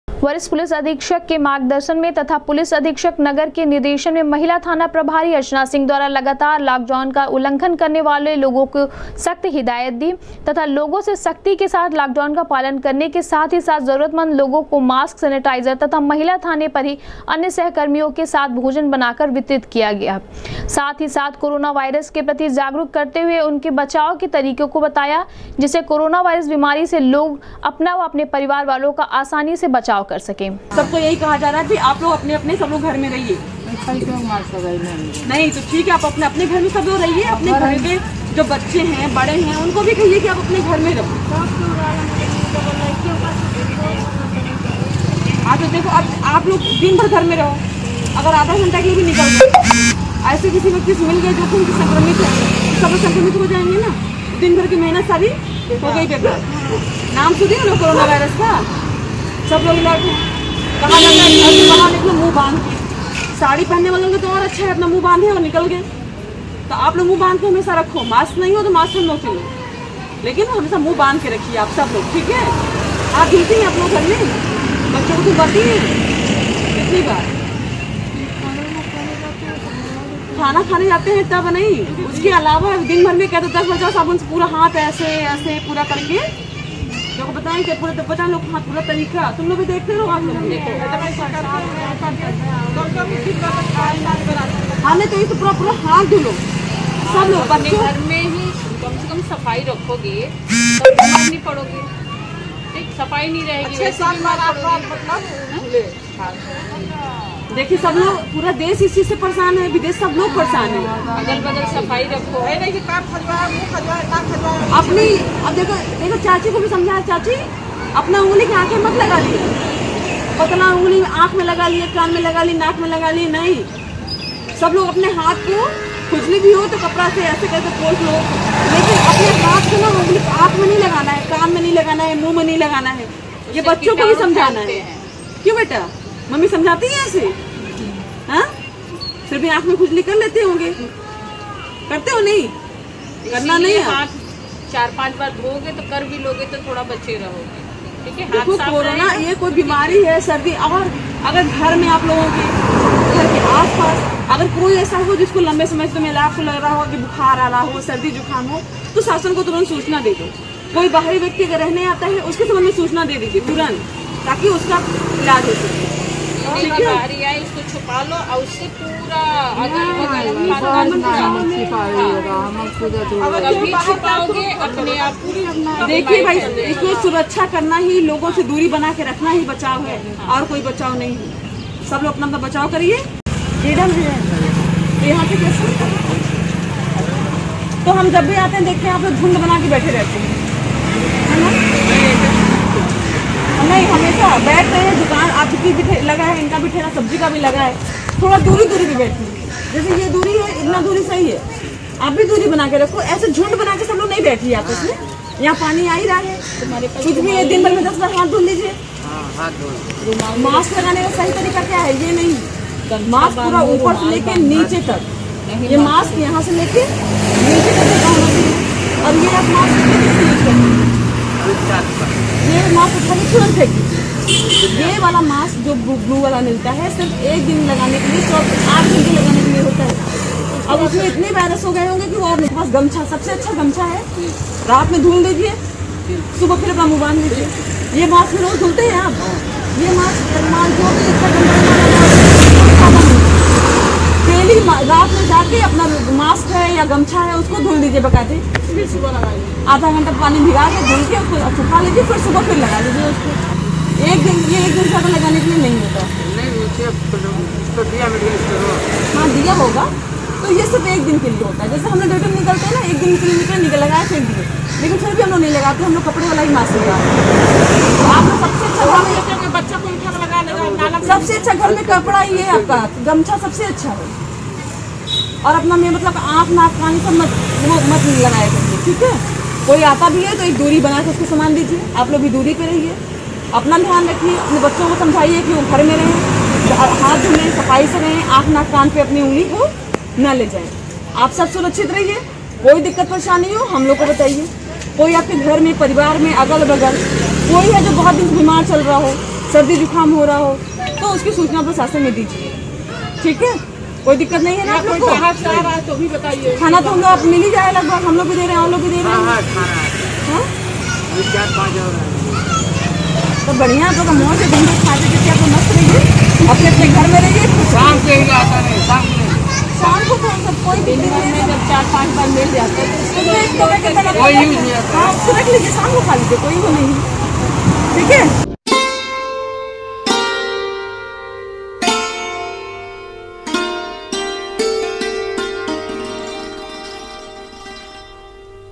लाकडाउन का उल्लंघन करने वालों के साथ सख्ती बरतने के साथ-साथ महिला थाना प्रभारी अर्चना सिंह ने
लाकडाउन का उल्लंघन करने वालों के साथ सख्ती बरतने के साथ-साथ महिला थाना प्रभारी अर्चना सिंह ने लोगों को कोरोनावायरस के प्रति किया जागरूक